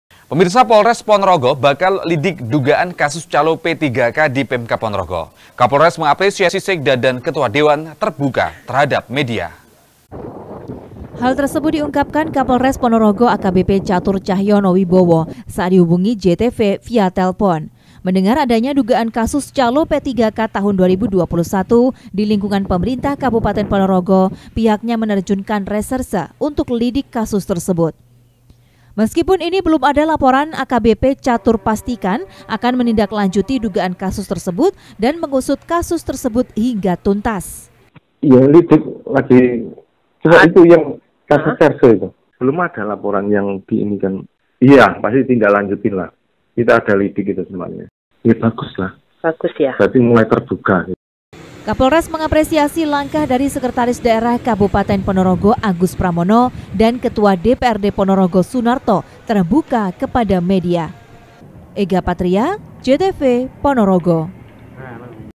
PONOROGO - Polisi Dalami Dugaan Kasus Calo PPPK Yang Seret Anggota Dewan Dan ASN
Polres ponorogo bakal lidik dugaan kasus calo pppk di pemkab ponorogo. Kapolres apresiasi sekda dan ketua dewan terbuka terhadap media.
Office : jl. Sarimulya no 47 Rejomulyo, Kartoharjo, Kota Madiun